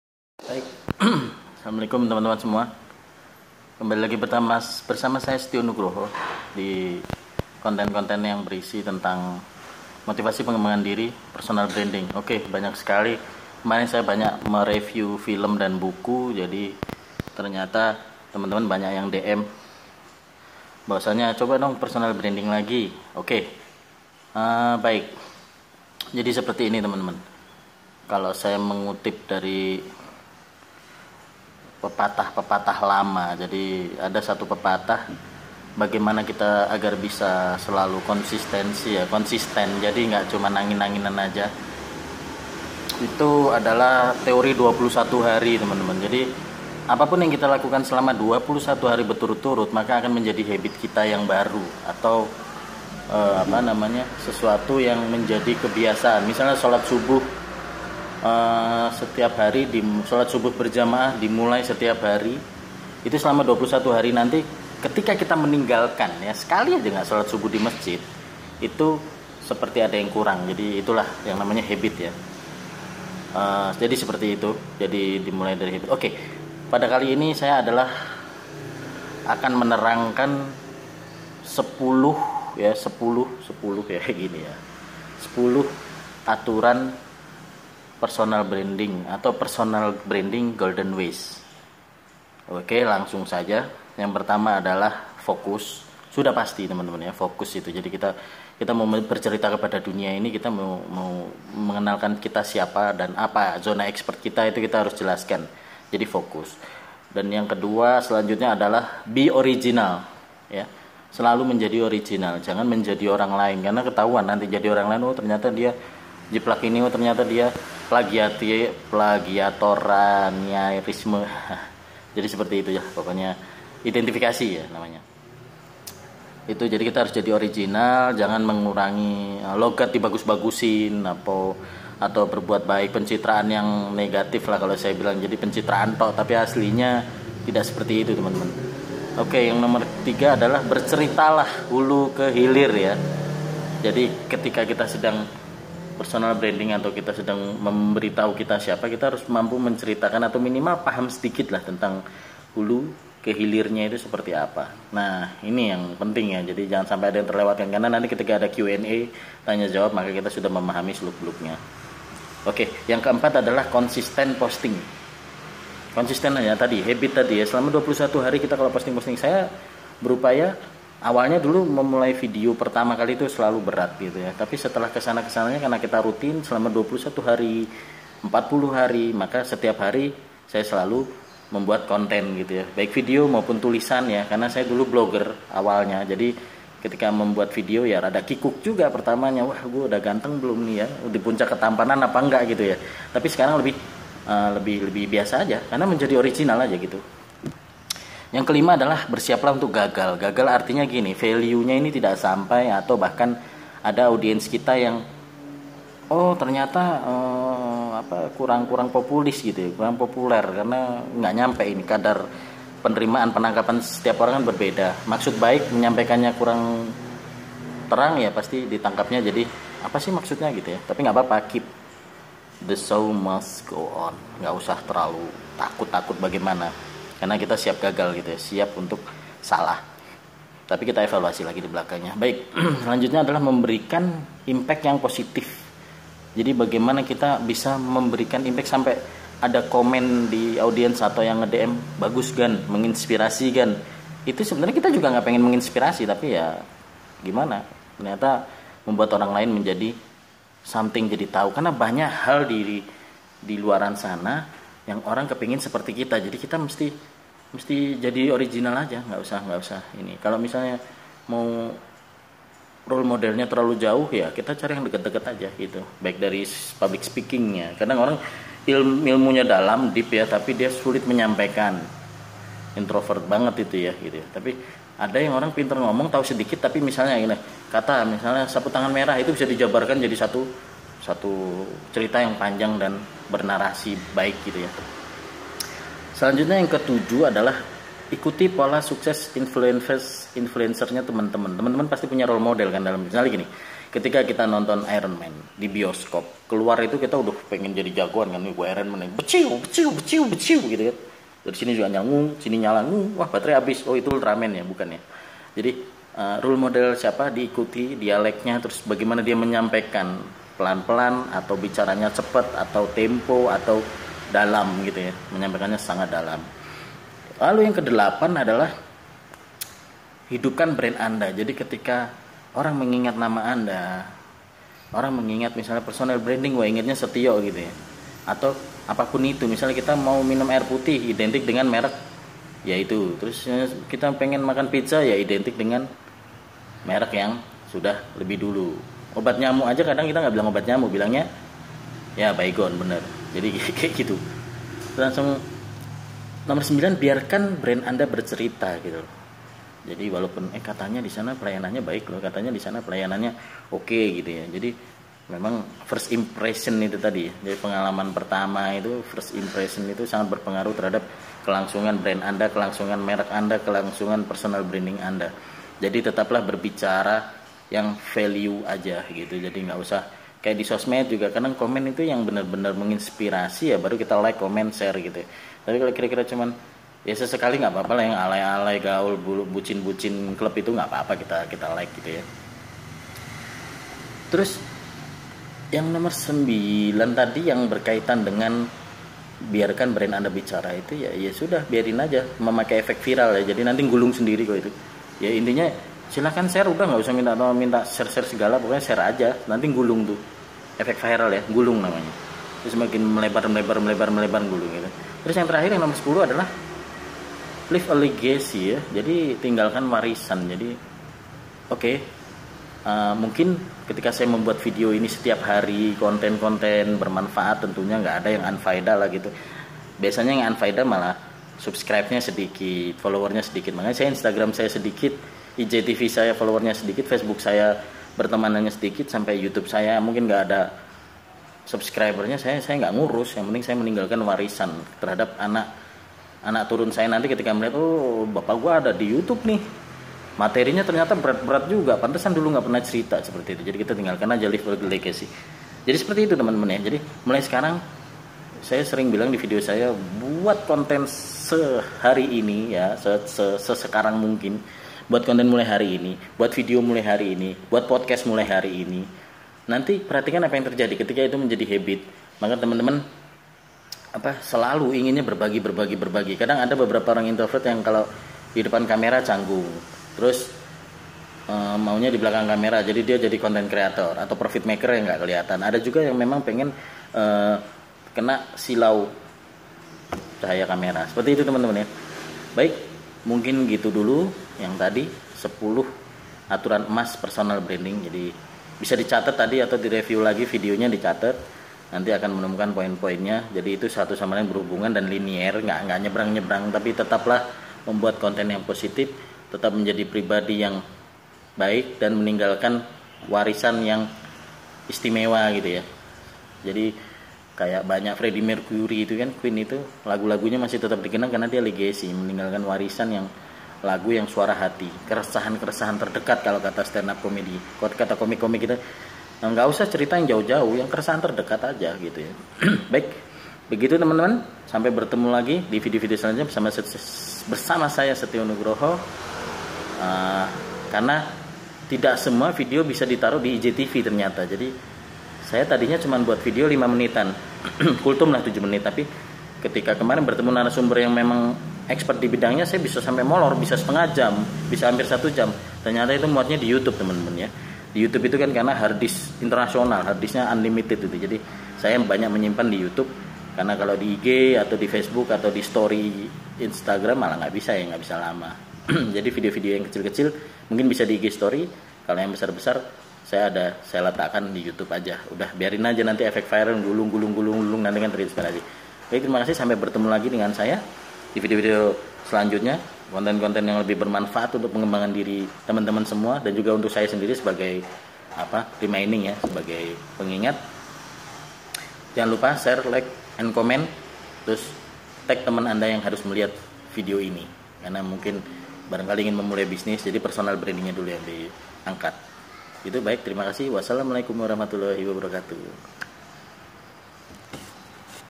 Tips Personal Branding yang Powerfull
TIPS PERSONAL BRANDING Sederhana untuk melejitkan Brand anda :
1. Fokus
2. ORIGINAL
3. Story telling
4. Konsisten Posting
5. Siap salah/gagal
6. Kreatif untuk memberikan IMPACT Positif ke audience anda
7. Ikuti pola sukses rule model anda
8. Hidupkan Brand anda
9. Biarkan Value anda yang menjadi pembicaraan audience anda
10. Leave a Legacy
Semoga bermangpaat.
Tetap kreatif, dan berkarya dalam situasi apapun, jangan tenggelam dalam kedukaan.. Show must go on dek...
Dari Sahabatmu.
Setio.